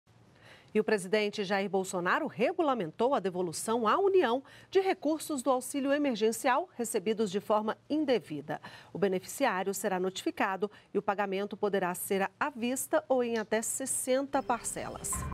Cidadania | Devolução do auxílio emergencial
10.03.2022 - O presidente Jair Bolsonaro regulamentou a devolução de recursos do auxílio emergencial recebidos de forma indevida. O beneficiário será notificado e o pagamento para União poderá ser à vista ou em até 60 parcelas.